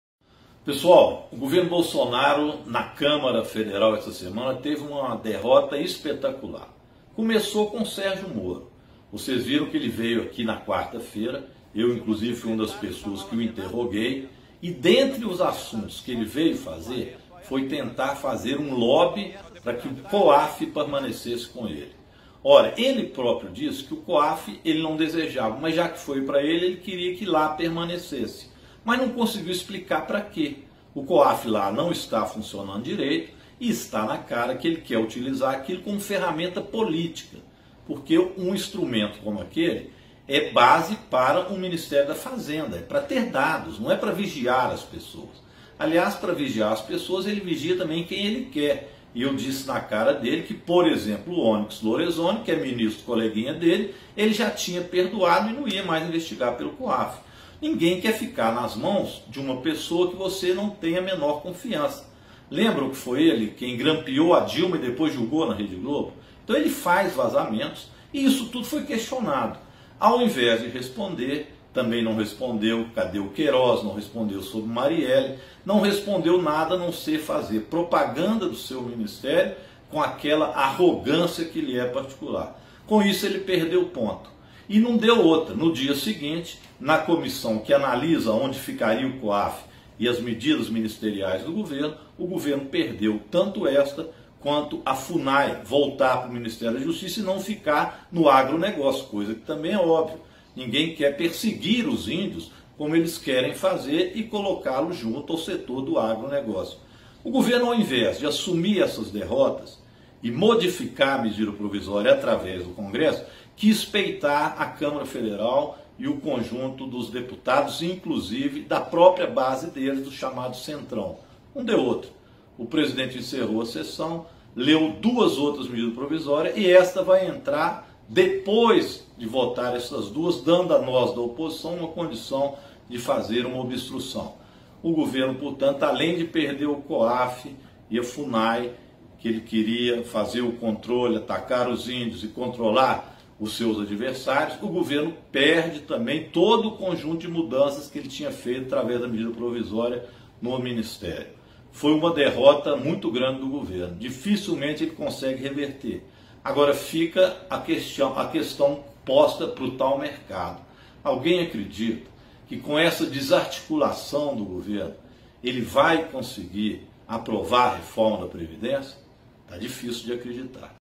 TRAPALHADA DE MORO EXPLICA DERROTA DO GOVERNO NO CONGRESSO
Neste vídeo de 3min, um resumo deste 9 de maio no Congresso, com derrotas significativas do governo Bolsonaro. O deputado federal Rogério Correia (PT-MG) explica lembrando que a perda nas votações do Coaf e Funai, para citar dois exemplos, começou no dia anterior, com o desastre que foi a participação de Sergio Moro na Câmara. Assista e compartilhe!